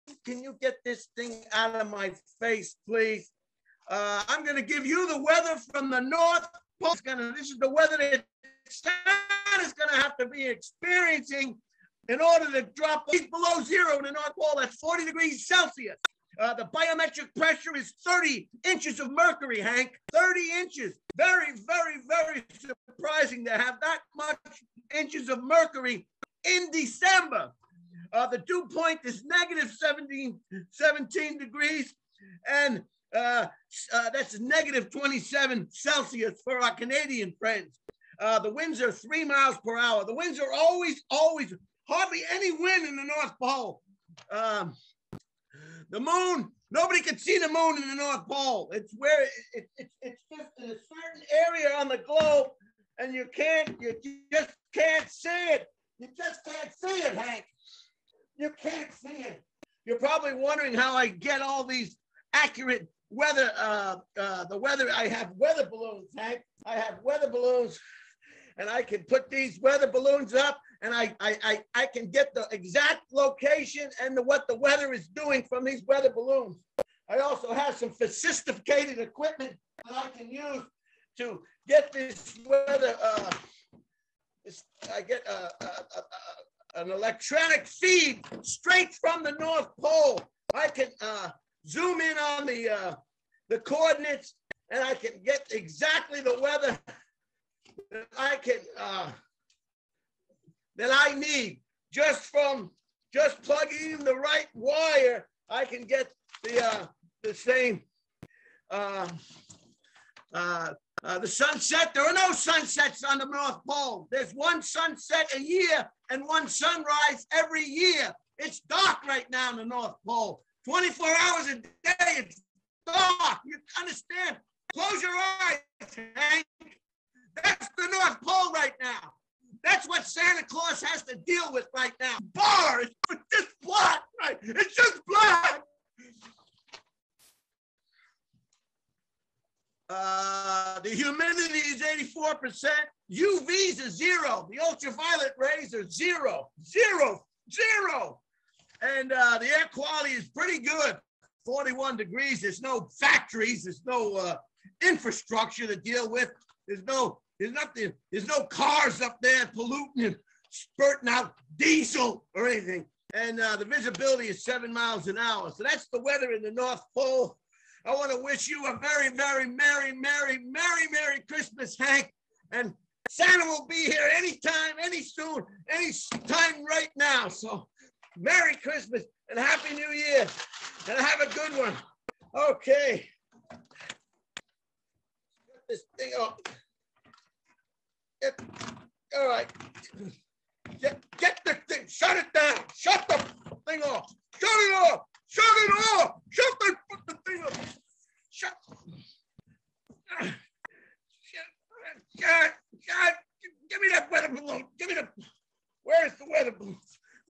Meteorologist Brian Holtzman From the North Pole
And now our personal Meteorologist Brain Holtzman presents the forecast at the North Pole with bad internet connectivity.